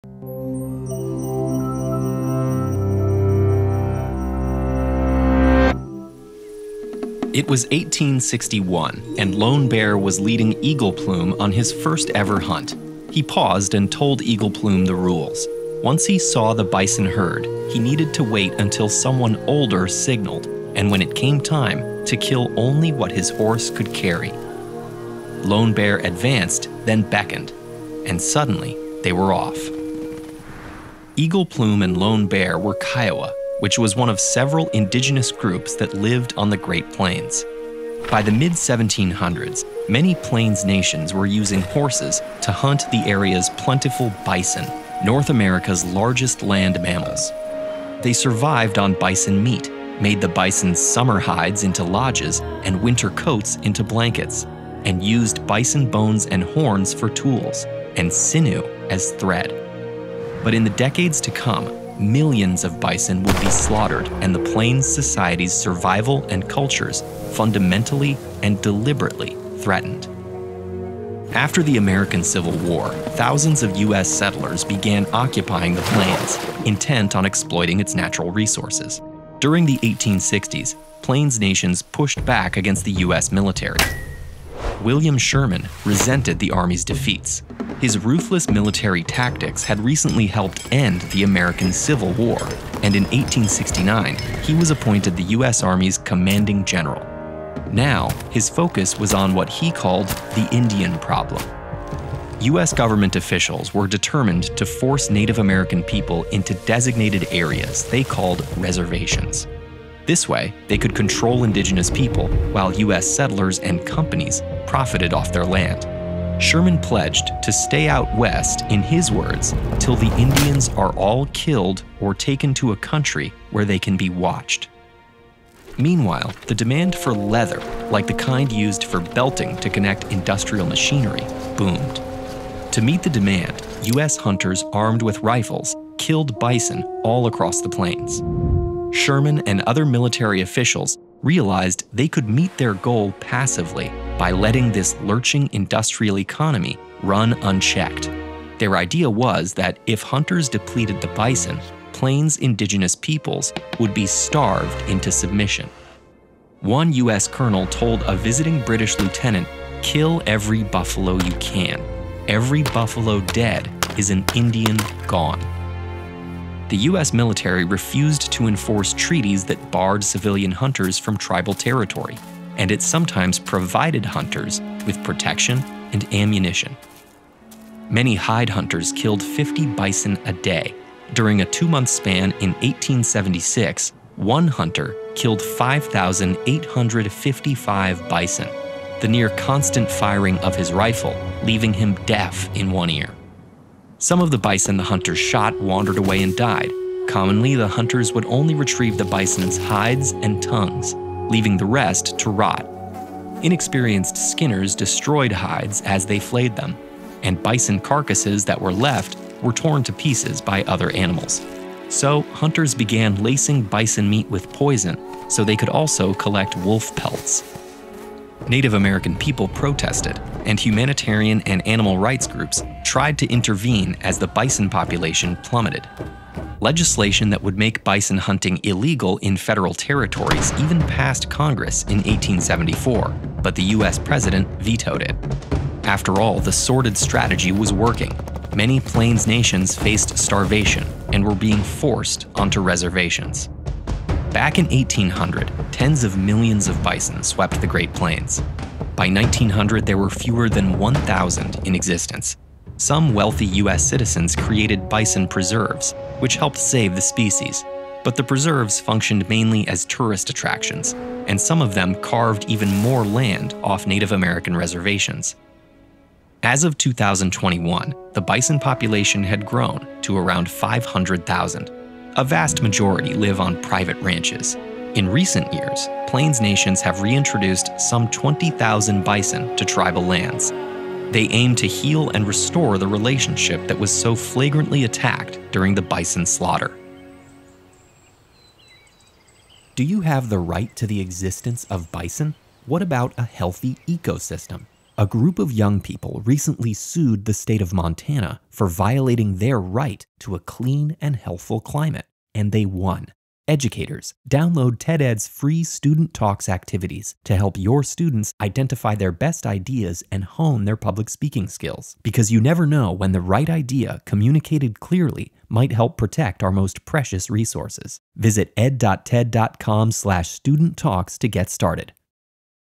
What killed all the bison? - Andrew C. Isenberg
Explore how the US government hunted bison to near-extinction in the 1800s to force Native Americans onto reservations.

By the mid-1700s, many Plains nations survived on North America’s largest land mammals: bison. They ate its meat, made the hides into winter coats and blankets, and used the bones and horns for tools. But in the following decades, millions of bison would be deliberately slaughtered, threatening the survival of Plains societies. Andrew C. Isenberg shares what led to the animal's near-extinction.

Lesson by Andrew C. Isenberg, directed by Rémi Cans, Atypicalist.

Thank you so much to our patrons for your support! Without you this video would not be possible!